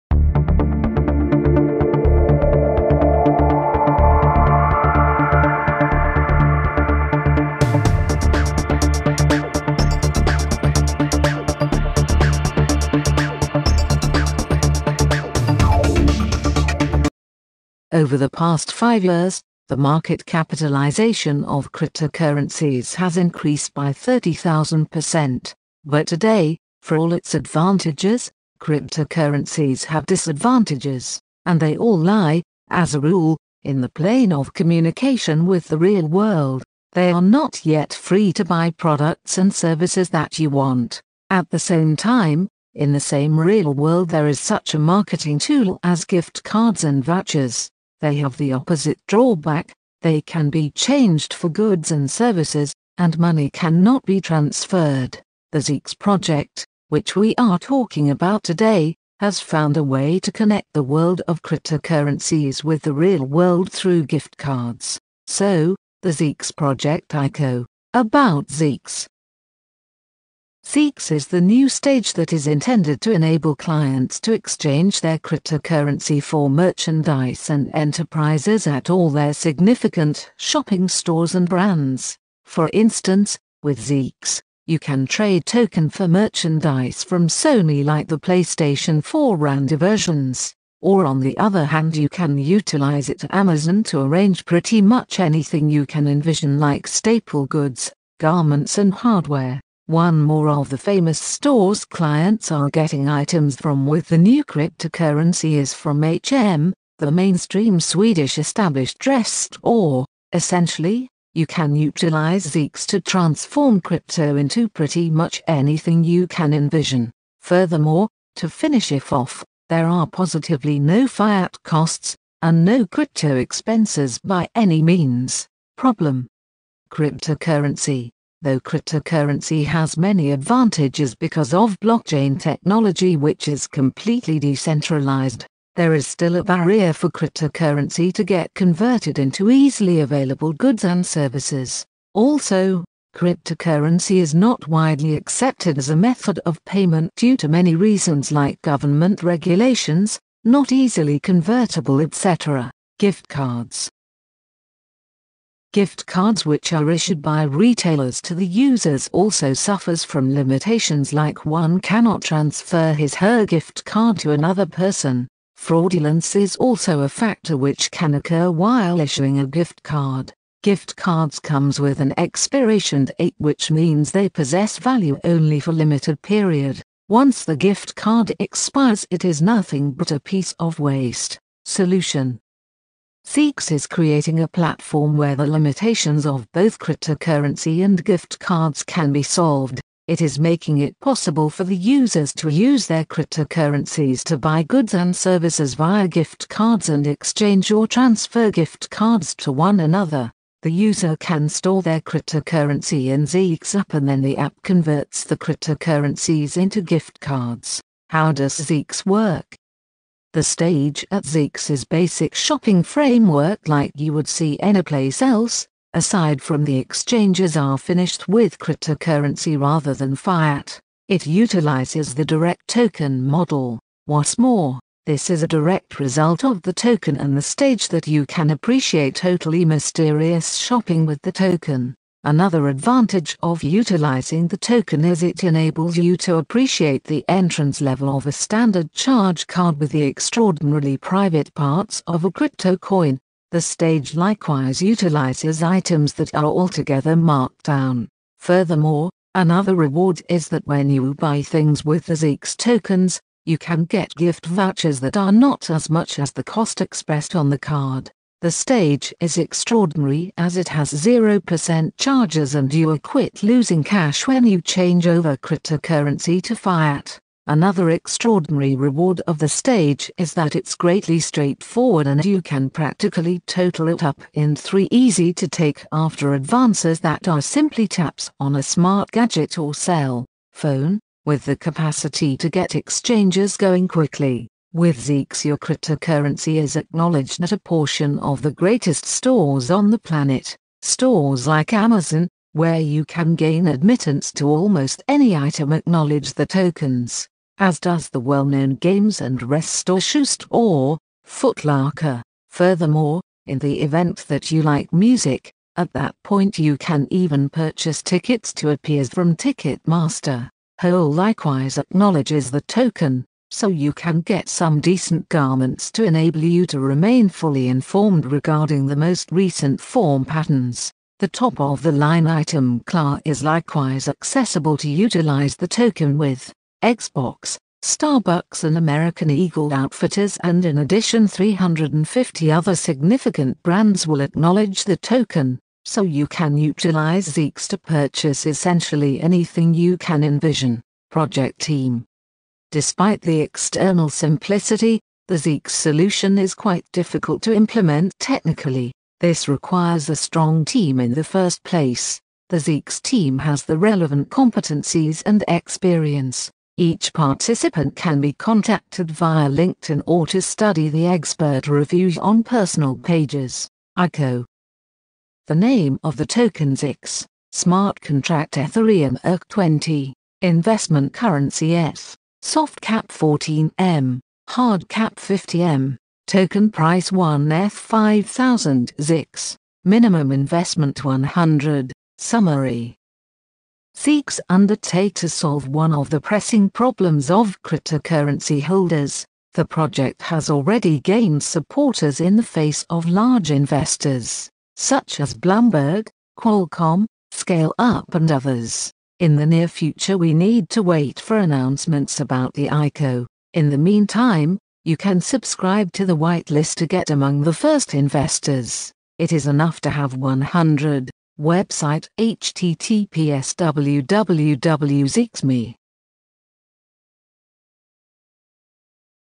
Zeex - Decentralized Crypto Shopping
Zeex is the new stage that is intended to enable clients to exchange their cryptocurrency for merchandise and enterprises at all their significant shopping stores and brands. For instance, with Zeex, you can trade tokens for merchandise from Sony like the Playstation 4 and diversions. Or on the other hand you can utilize it Amazon to arrange pretty much anything you can envision like staple goods, garments and hardware. One more of the famous stores clients are getting items from with the new cryptocurrency is from H&M, the mainstream Swedish established dress store. Essentially, you can utilize Zeex to transform crypto into pretty much anything you can envision. Furthermore, to finish if off, there are positively no fiat costs, and no crypto expenses by any means.